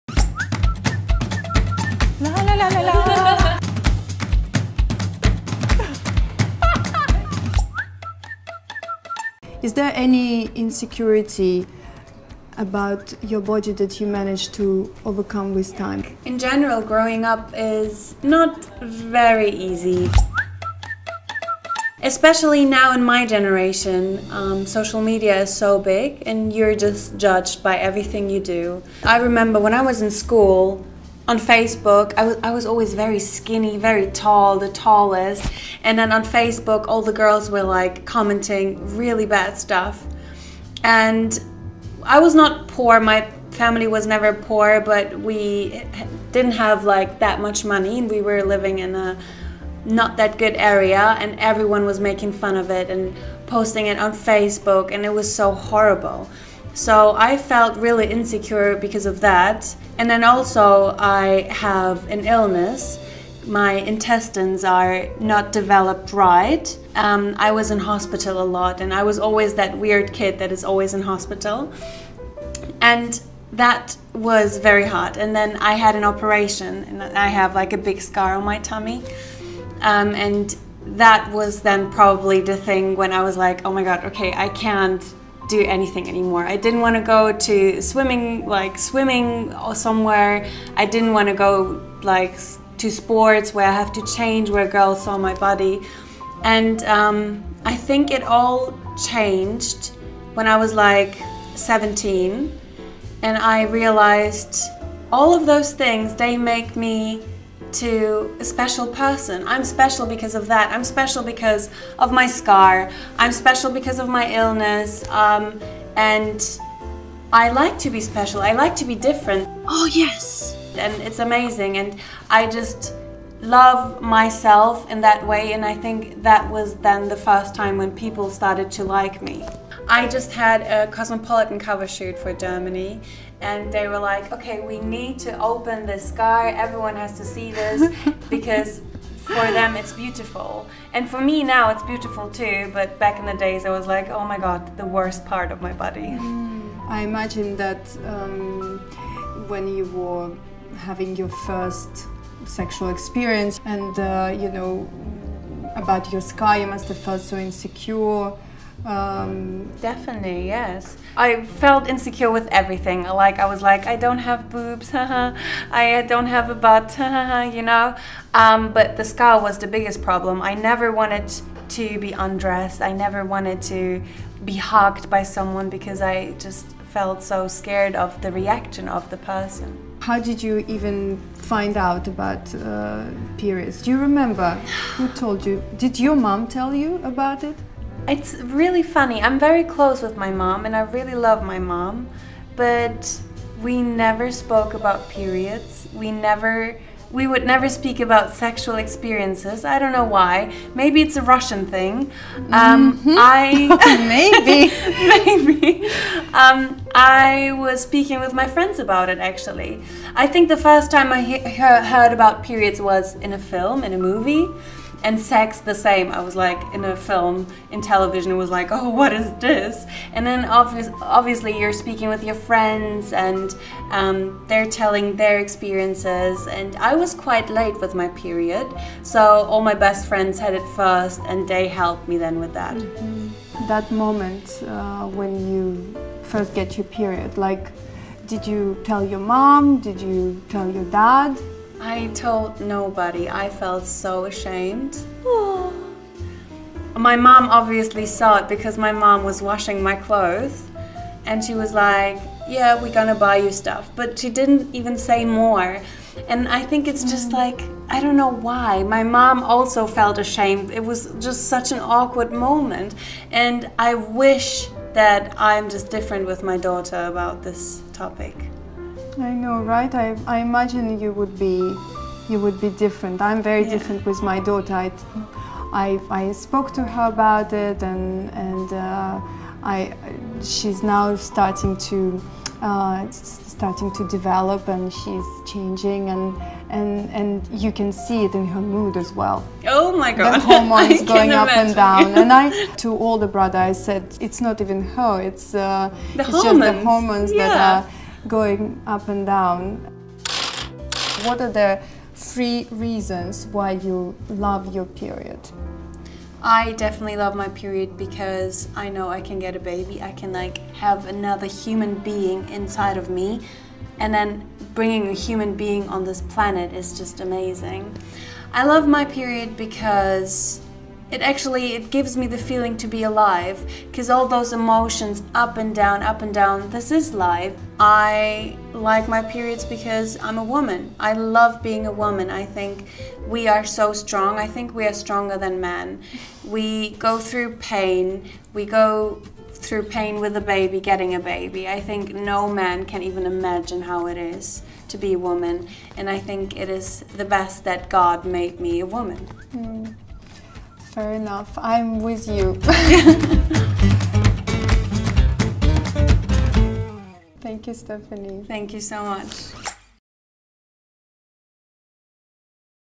Let’s Talk About It. Period. with Stefanie Giesinger
Insecurities, challenges, pain? Time to speak up! Watch the video.

Let’s Talk About It. Period is a worldwide awareness campaign to stop the stigma surrounding women’s health, particularly menstruation.